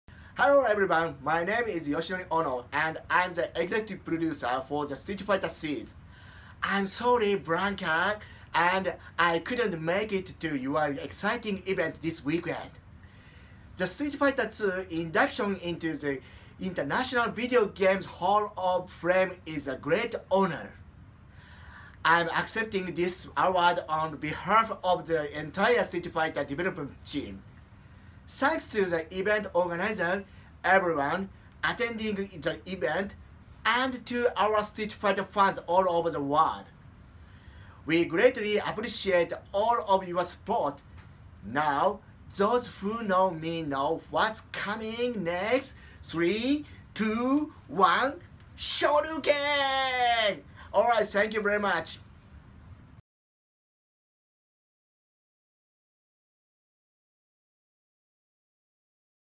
Yoshinori Ono: IVGHOF 2019 "1990's Era Game" - Street Fighter
Yoshinori Ono official acceptance speech for Street Fighter Franchise.